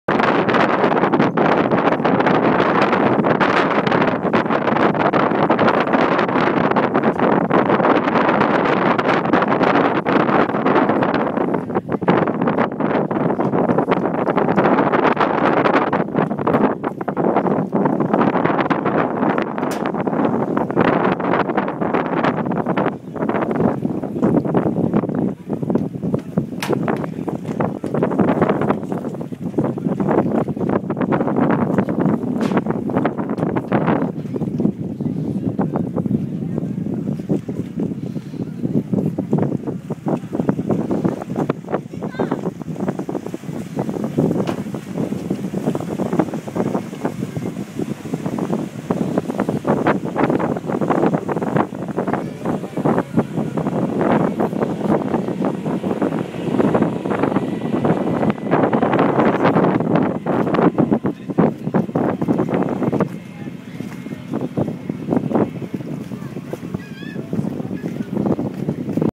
28-04-2013 sortie en mer à bord d'un SEMI rigide des comman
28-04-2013 sortie en mer à bord d'un SEMI rigide des commandos marine, 39 Nœuds atteint, trop trop bien.
Ce SEMI rigide, a été lancé de 1500 mètres de haut d'un avion sur l'eau pour tester sa solidité.